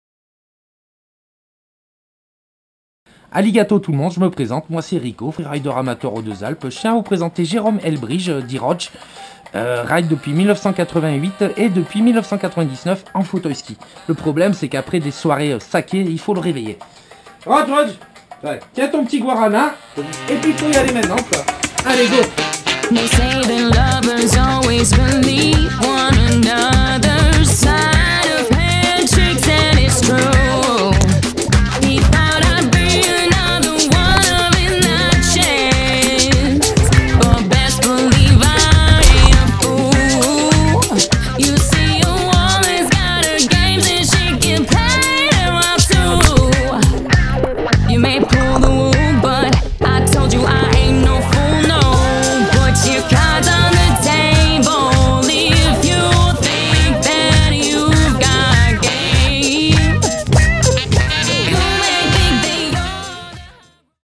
J. Elbrycht - Julbo White Session Video Contest 2013 Sitskiing
jerome Elbrycht video pour Julbo White Session Fly to Japannnn!! Ok la video est postée grave a la bourre mais désolé ... j'etais sur les lattes... Mais vous avez vu, je me suis mis direct au montage avec la fin fatidique du concours. :)